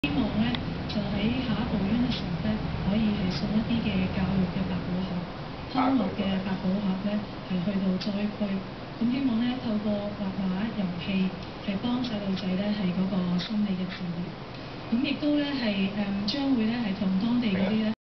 U2 Cafe Earthquake Donation on TVB1
On May18, 2008.  U2 Cafe donated $1000.00
to the earthquake victims of China